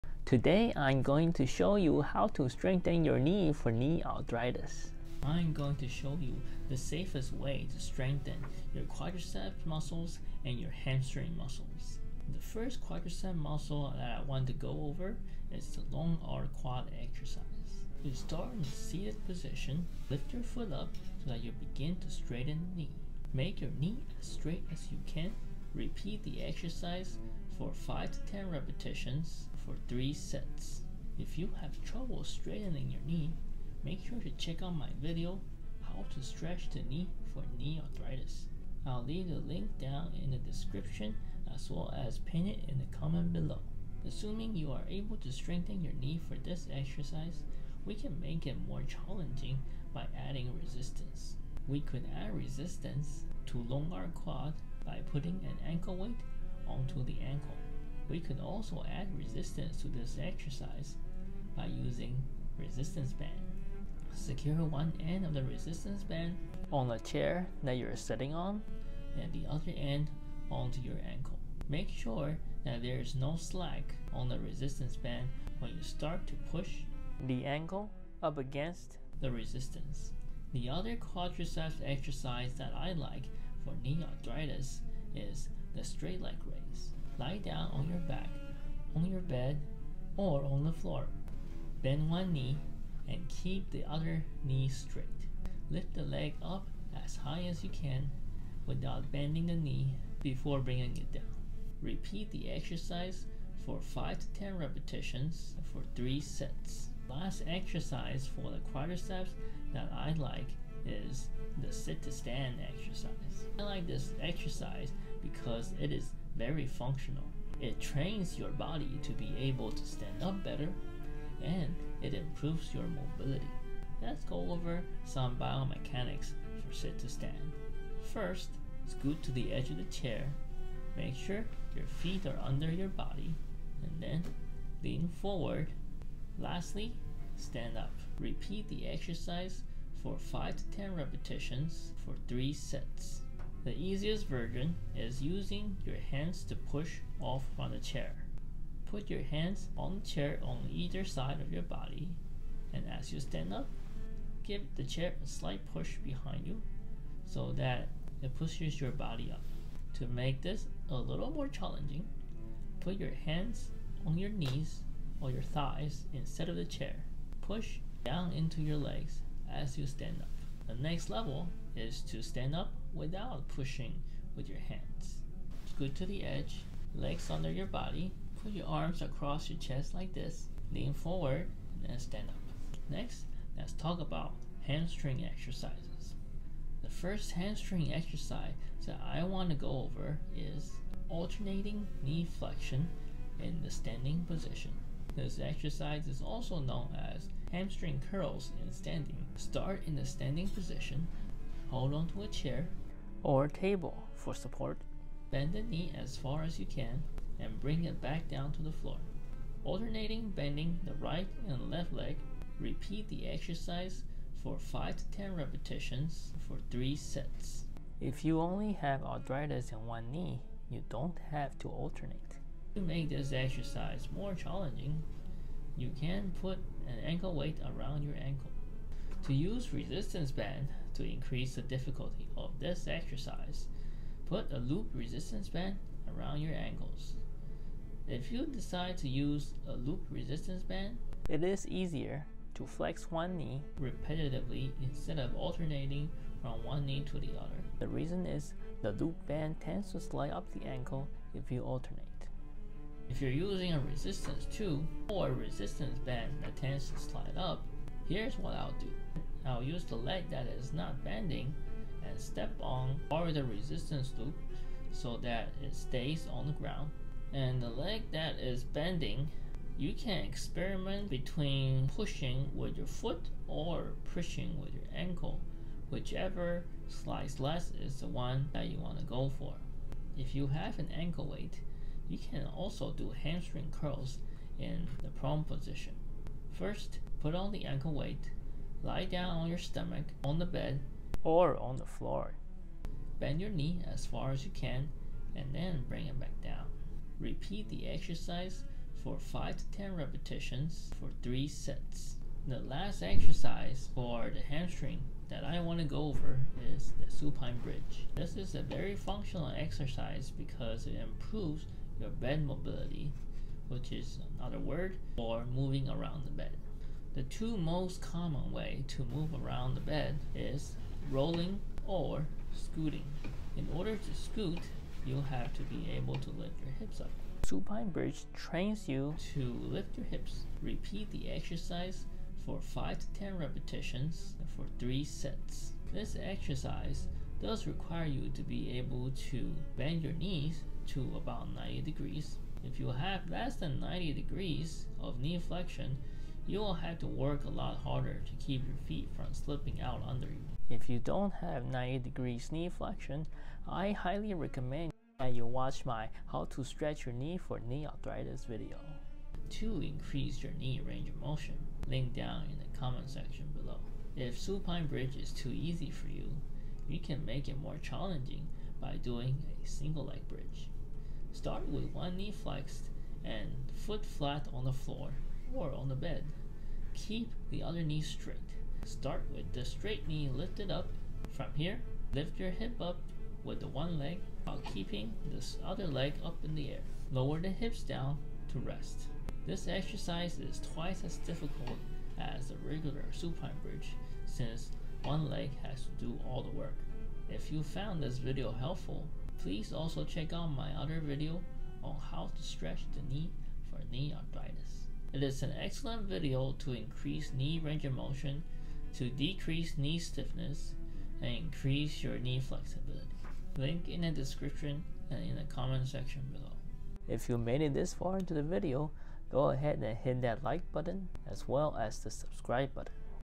How to Strengthen the Knee for Knee Arthritis
Today I’m going to show you how to strengthen the knee for knee arthritis. If you have knee arthritis, the cartilage in your knee is wearing away, causing the joint to be stiff. We would like the knee to be surrounded by as much muscle as possible to support the knee joint. Theses exercises are also excellent for people who have meniscus tears or meniscus injuries.

The two muscle groups that are the most important are the quadriceps muscles and the hamstring muscles.

To strengthen the quadriceps muscles in a way that does not wear away more cartilage, I like to start with non-weight bearing exercises (exercises that does not involve standing).

The first quad exercise is the long arc quads. We can start in the seated position. Bring the knee into full extension to strengthen the quads. To make this exercise more challenging, we can add resistance through either ankle weight or resistance band.

Adjustable ankle weight
Resistance band

The second quad exercise is the straight leg raise. We can start in the supine position (lying down on your back, facing up). Keep the knee straight and lift the leg up and down. Again you can increase the difficulty by using an ankle weight.

Lastly, I like to throw in a transition exercise like sit to stand. This is an exercise that transitions from sitting to standing. This is a transfer exercise that is very functional. It trains your legs to be more efficient in standing up.

The first hamstring exercise is alternating knee flexion in the standing position, or just hamstring curls in standing. Start in the standing position, alternating bending the knees one at a time. Hold onto a chair or table for balance. You can increase the difficulty by adding resistance, with either ankle weight or resistance band.

You can also do hamstring curls in the prone position (lying down on your stomach, facing down). You can also use the ankle weight in this position.

The second hamstring exercise I like is the supine bridge. This is also a very function exercise. It helps you to become more efficient in bed mobility (moving around on your bed). This exercise is also called the hip thrust. You can make this exercise more challenging by keeping one leg up in the air and using the other leg to bridge (lift your hips up).

Timecodes:
0:00 - Introduction
0:13 - Long Arc Quads
1:24 - Straight Leg Raise
1:52 - Sit to Stand
3:11 - Standing Hamstring Curls
5:05 - Supine Bridge

All footages used in this video are either original or obtained via Storyblocks.

Tips for knee arthritis flare ups:
Consult with a local physical therapist. Your physical therapist could teach you which exercise is appropriate for you and customize a program for you. Please know that a good therapy program doesn’t just include strengthening. it also includes stretching, manual therapy, and pain control.

If your knee is flared up, ice the knee to decrease inflammation. Make sure there is a layer of clothing between the ice pack and your skin so you don’t get frost bite.

After doing knee exercises, if there is any pain, ice the knee again. Make sure there is a layer of clothing between the ice pack and your skin so you don’t get frost bite.

It is a good idea to massage and stretch the knee before you stretch it. Massaging the muscles around the knee helps to relax the muscles and warm up the muscles and the joint by increasing blood flow to them.